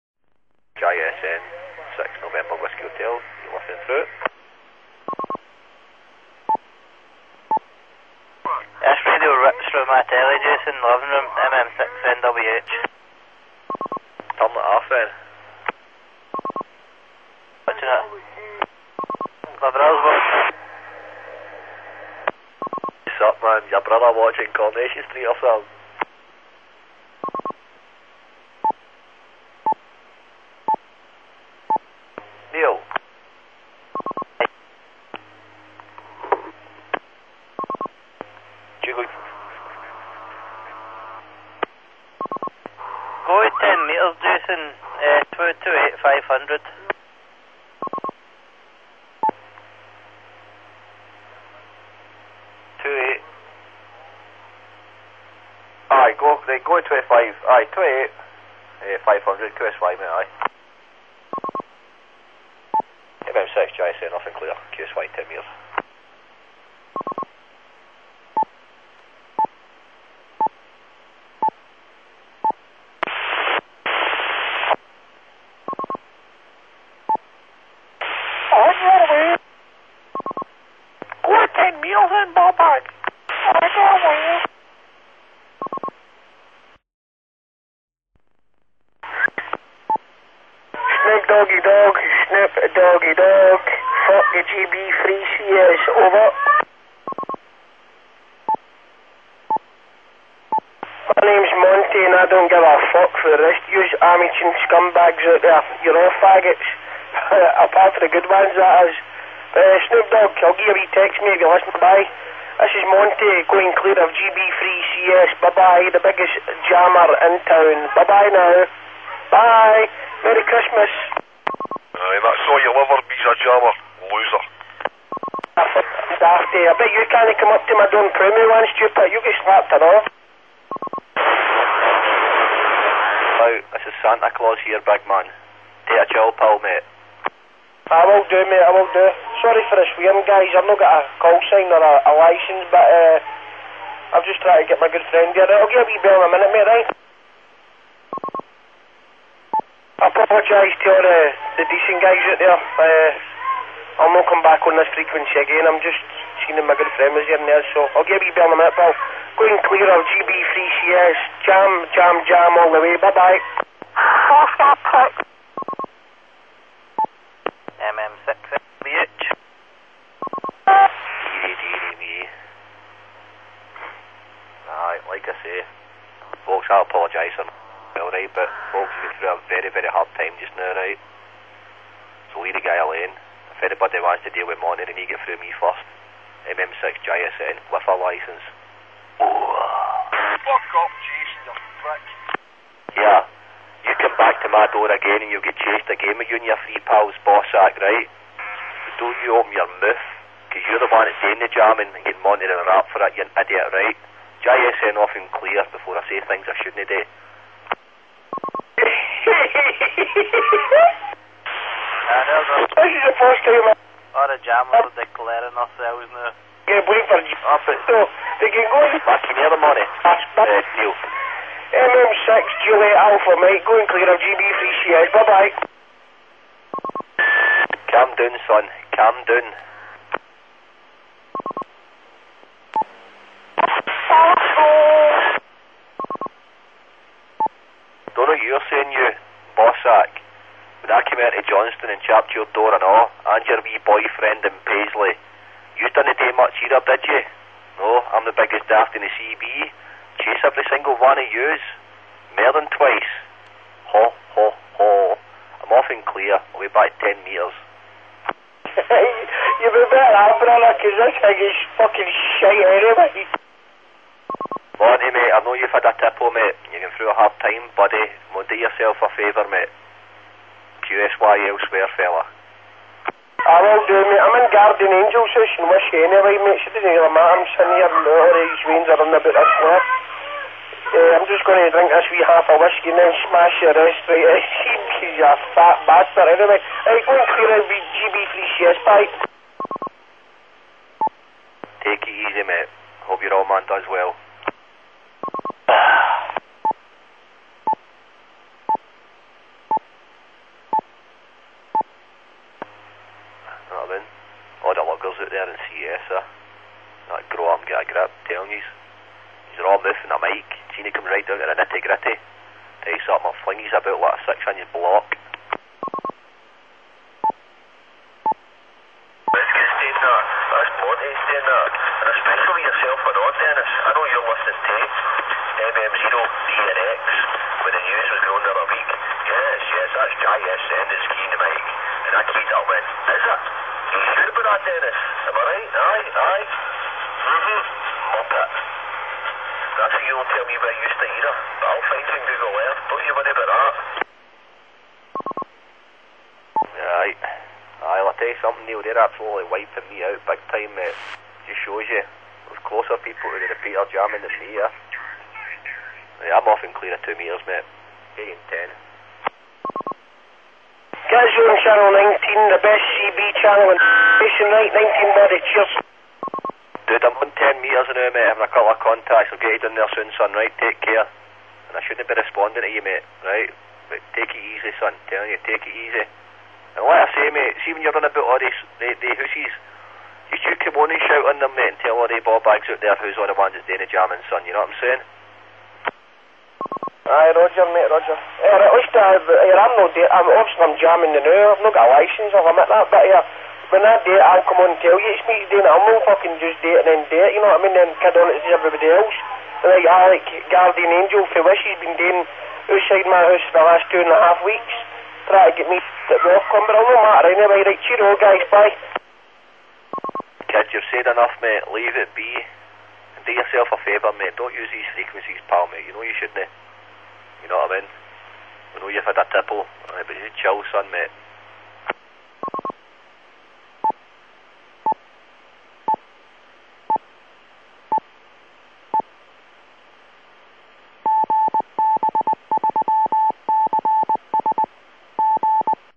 GB3CS the Central Scotland CB repeater Monday night PART 1
PART1 of 3
Recorded Monday 19th December 2011 around 7pm

MM6JSN & MM6NWH are on GB3CS when an unlicenced station 'Monty' comes onto the repeater. Foul language and everyone breaking licence conditions.